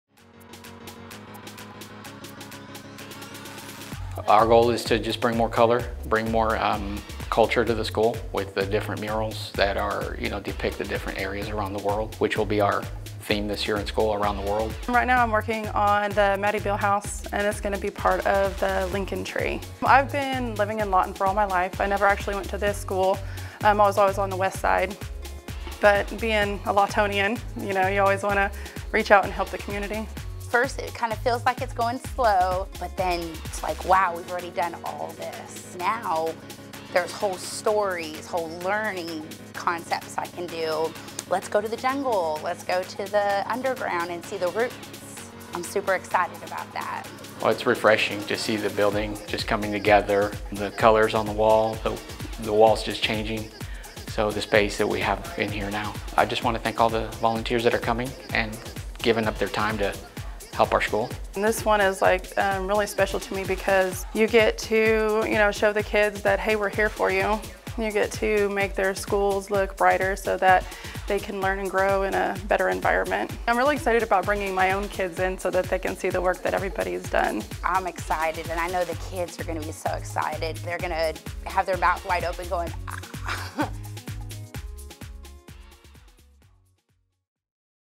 Lawton Public Schools: Lincoln Elementary Murals Update
Staff and volunteers are making tons of progress on their murals at Lincoln Elementary. Check out how far they've come since just last month!